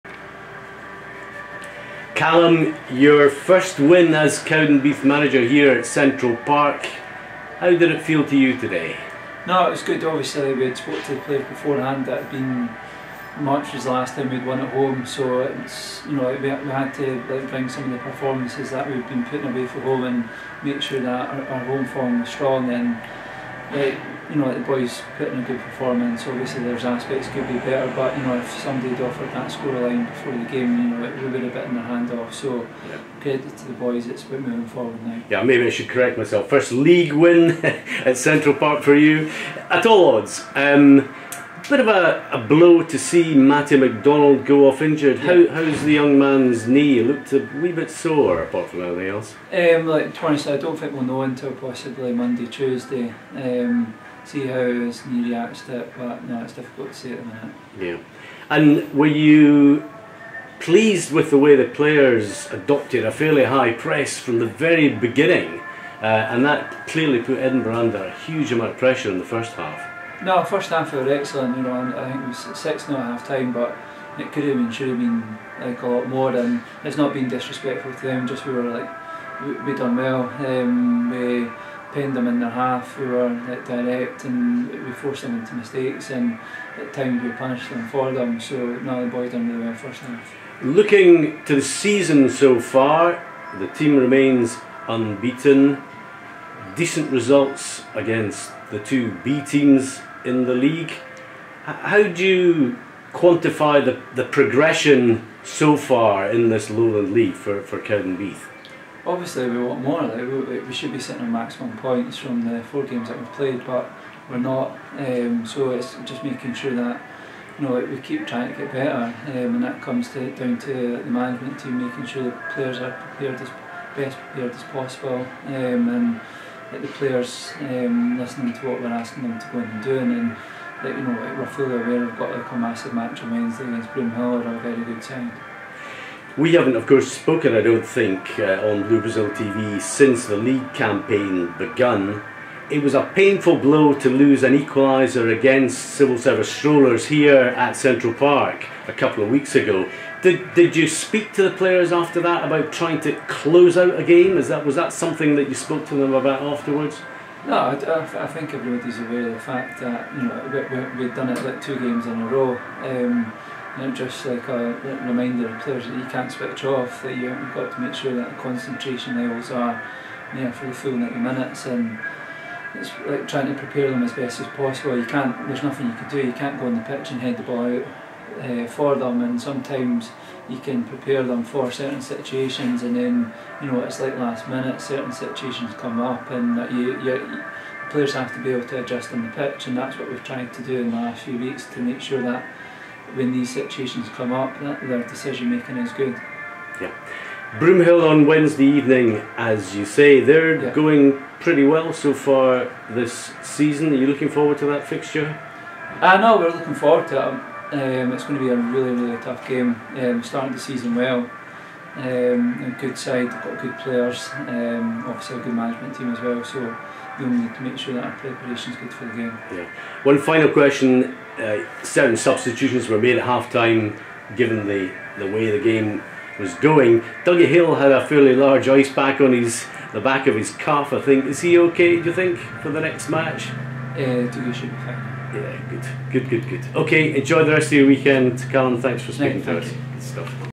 BBTV | Calum Elliot - Post Match v Edinburgh University (19/08/2023)
BBTV captured the thoughts of manager Calum Elliot after the 7-1 win over Edinburgh University on Saturday 19th August 2023.